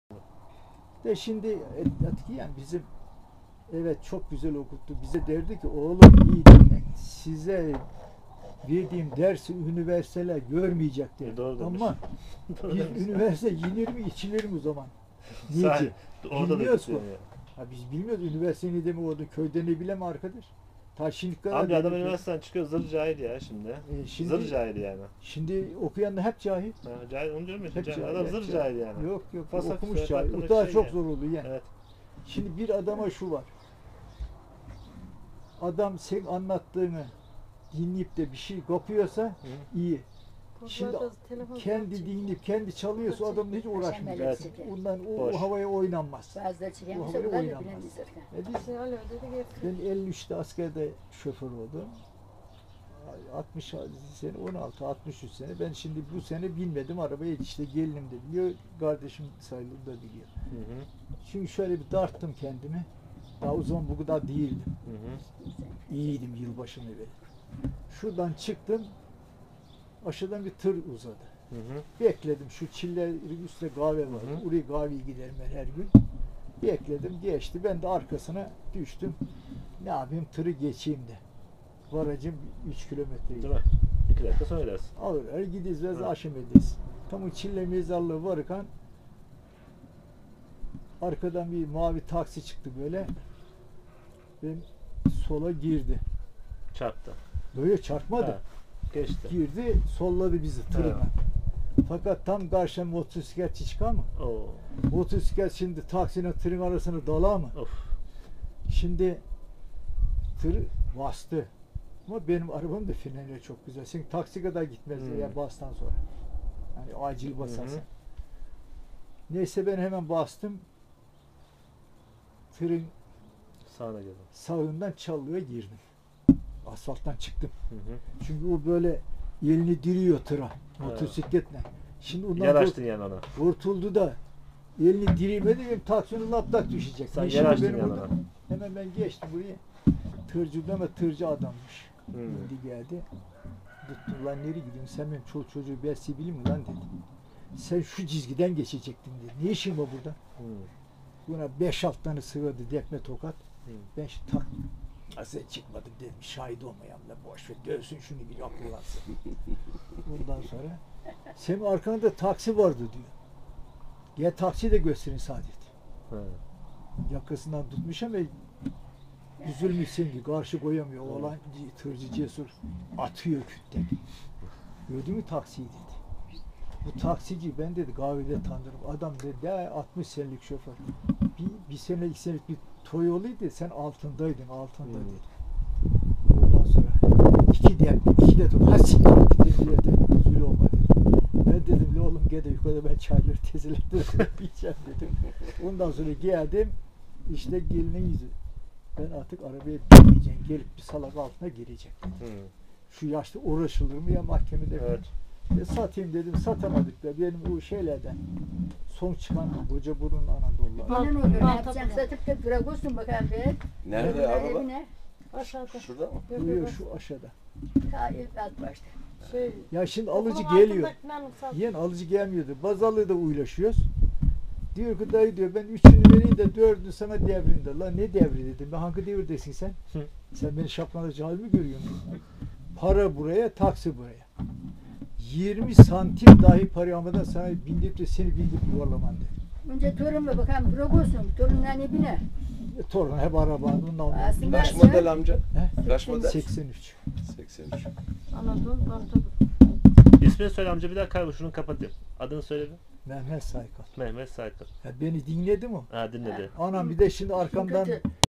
Mehmet amcanın şöförlüğü bırakma hikayesi
Mehmet amca eski zamanlardan ve araç kullanmayı neden bıraktığını anlatıyor.